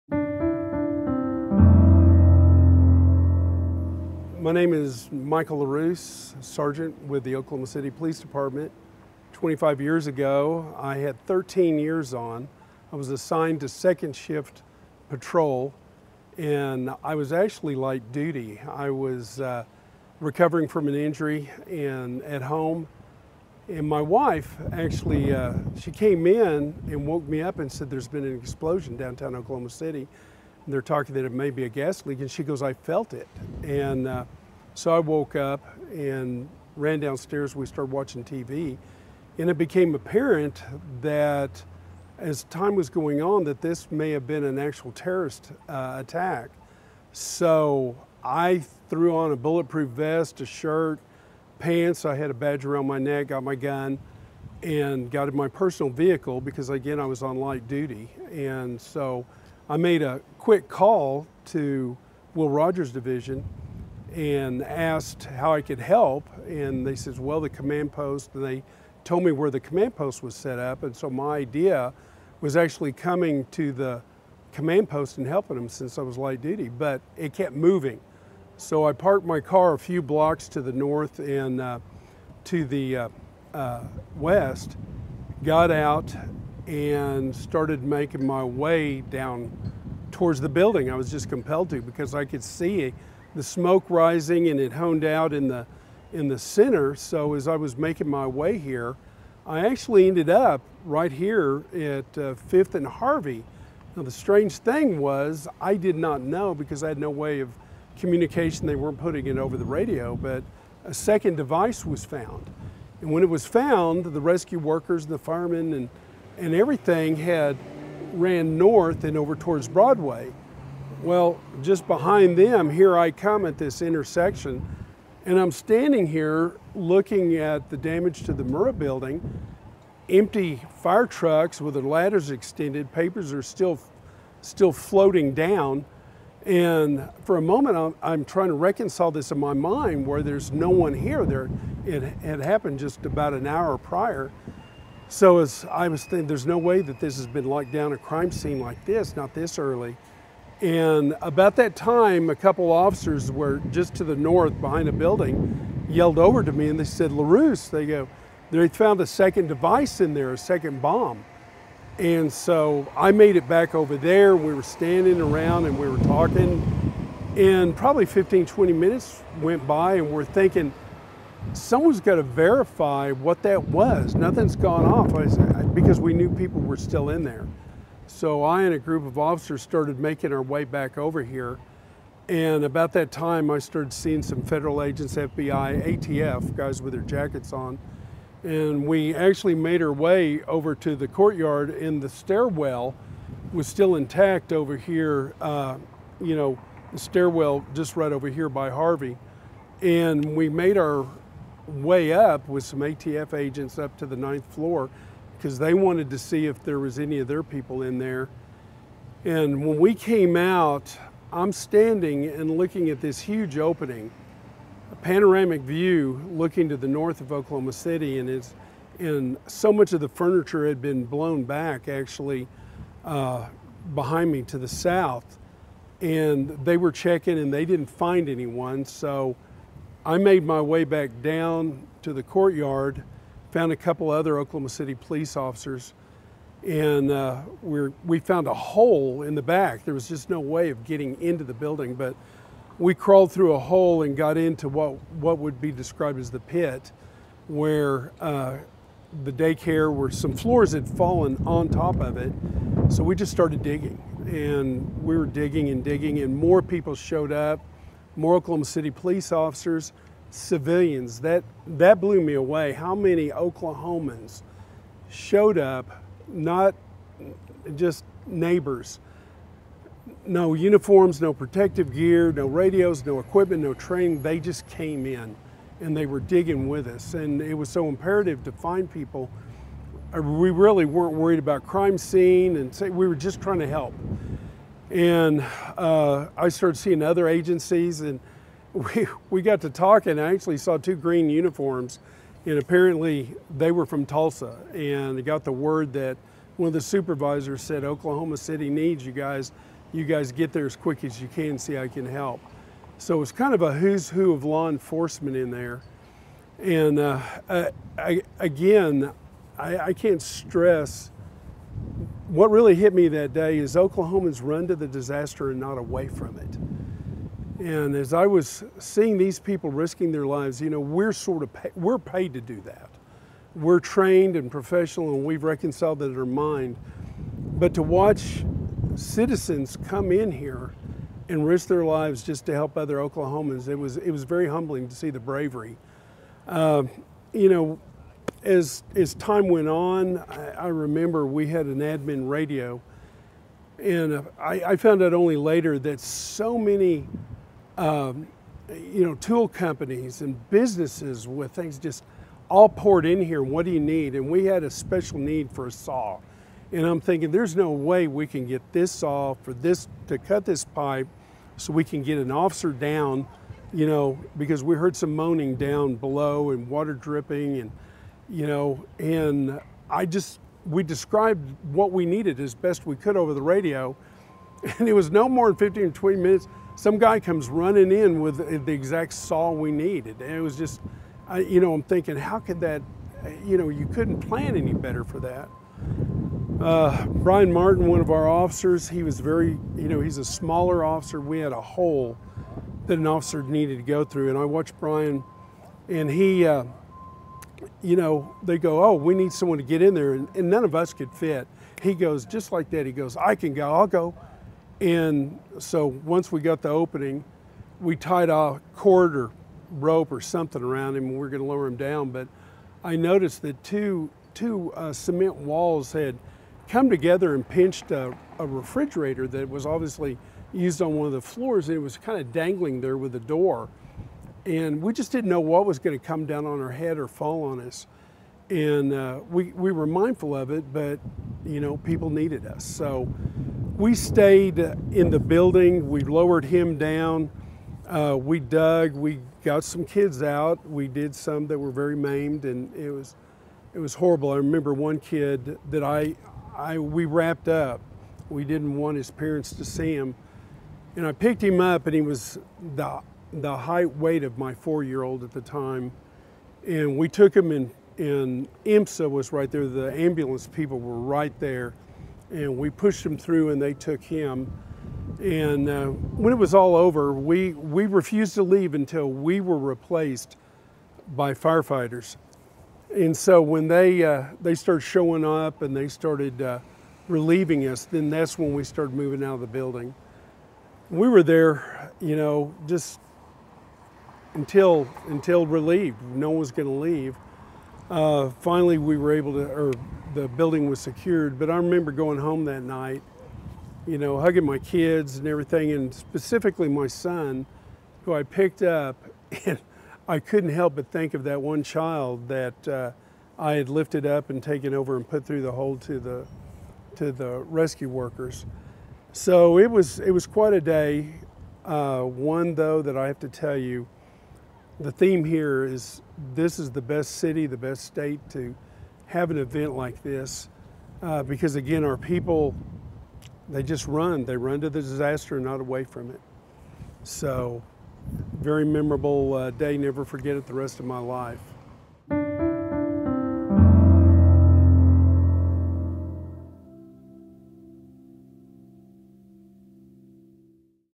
25 Years Remembered. MSgt Michael Loruse
MSgt. Michael Loruse remembers April 19, 1995